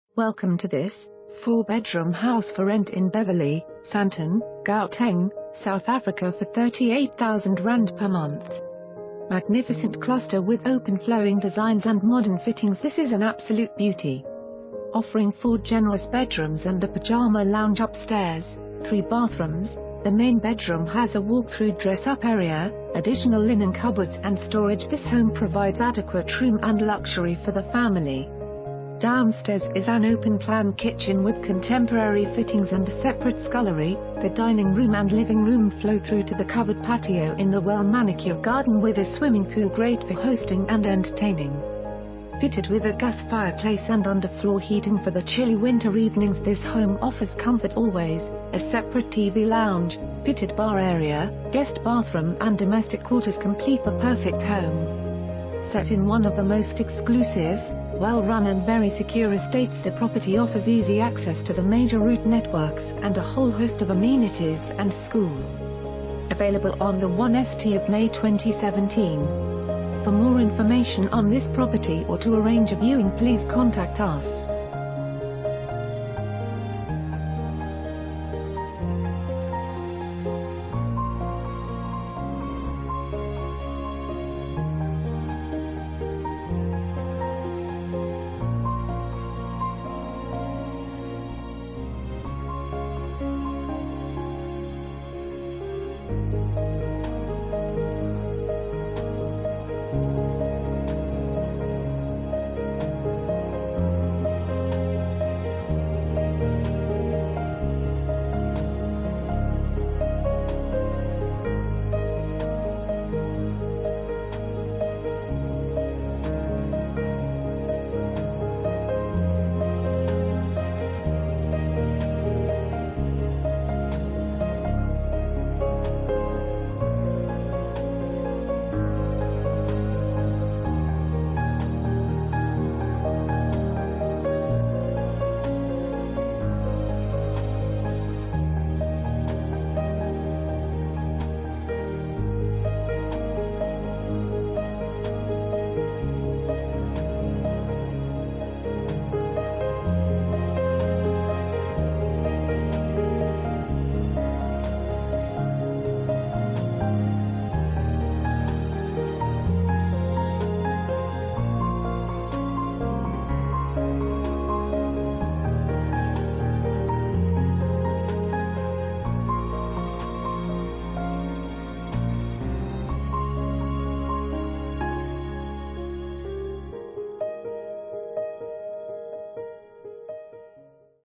4 Bedroom House For Rent in Beverley, Sandton, Gauteng, South Africa for ZAR 38,000 per month
Magnificent cluster with open flowing designs and modern fittings this is an absolute beauty. Offering 4 generous bedrooms and a pajama lounge upstairs, three bathrooms, the main bedroom has a walk-through dress up area, additional linen cupboards and storage this home provides adequate room and luxury for the family. Downsta...